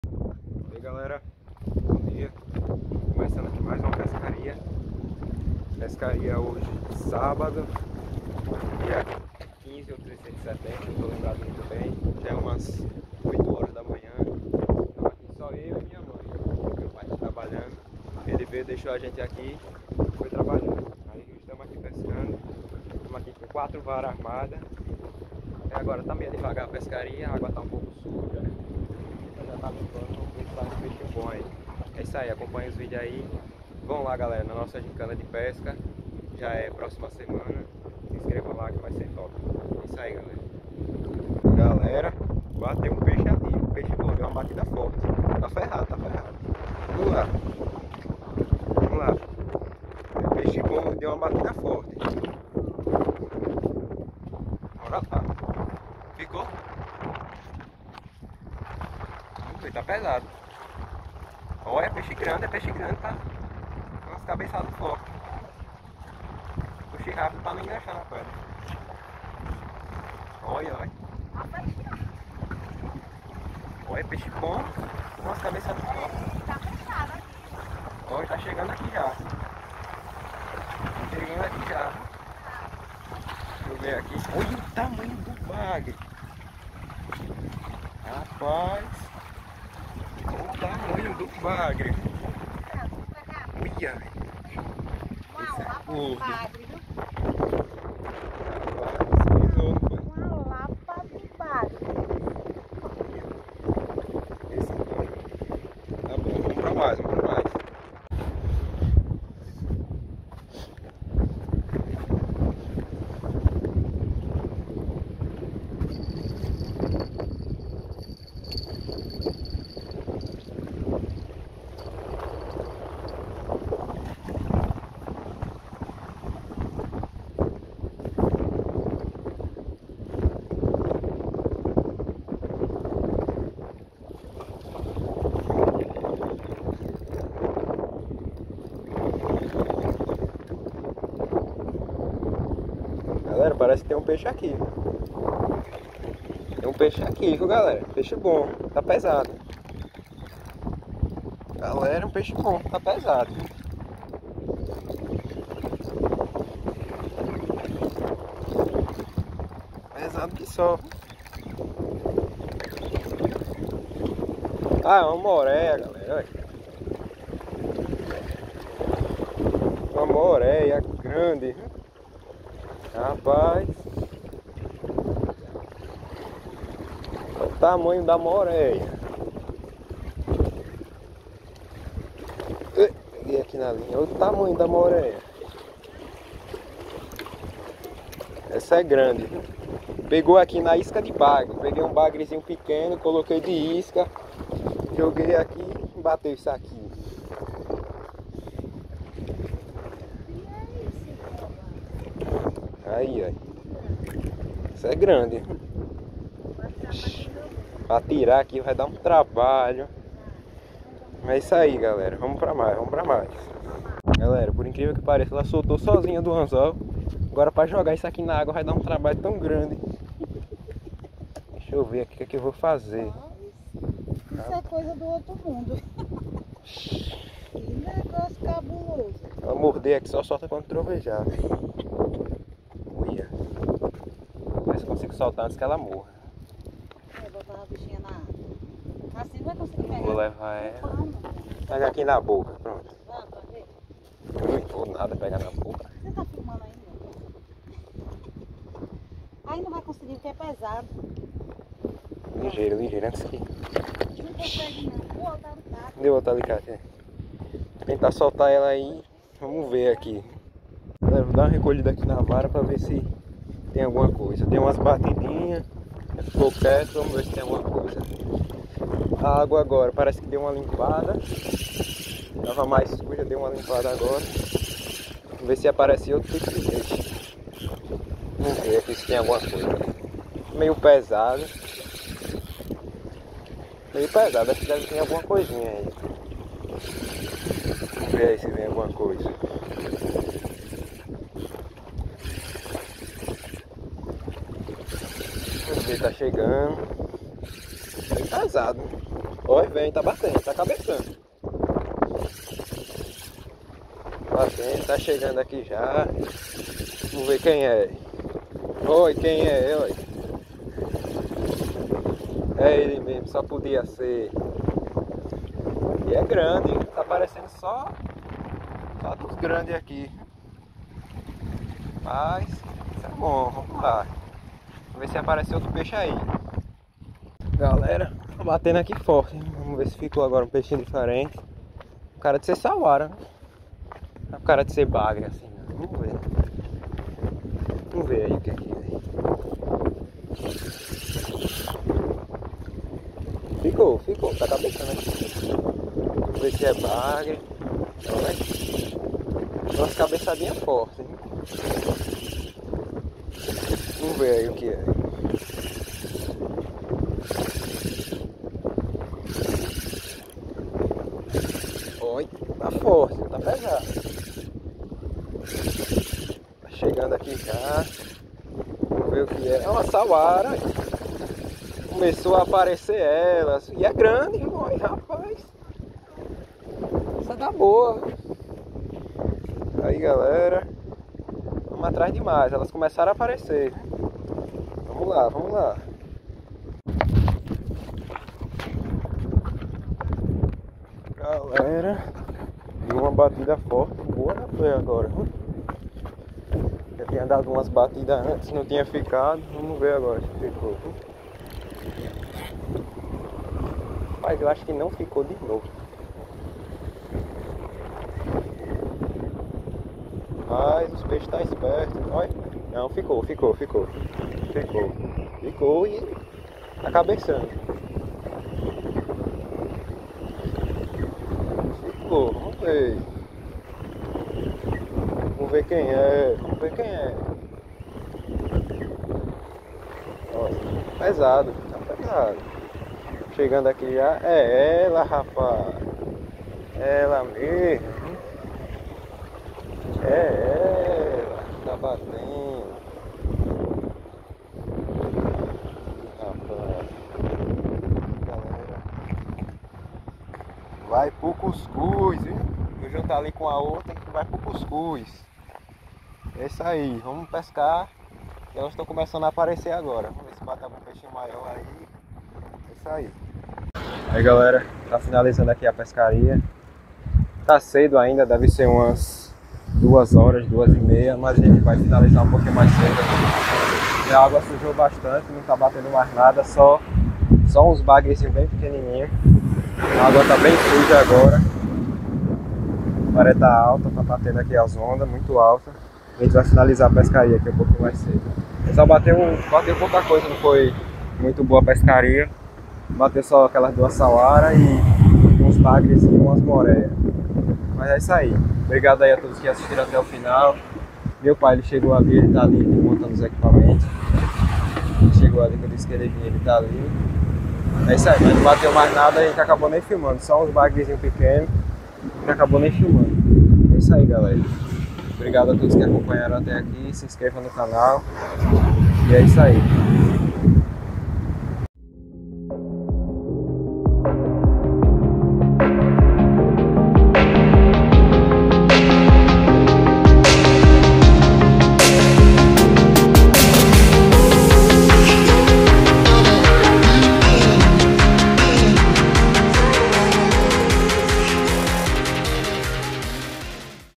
PESCARIA RÁPIDA SÓ EU E MINHA MÃE, VOCÊS NÃO VÃO ACREDITAR O QUE SAIU NESSA PESCARIA!!!
Fala galera mais uma pescaria nas pedras, pescaria com pouca ação mas bem produtiva!!

Inscrição Ou Mais Informações Sobre A Gincana-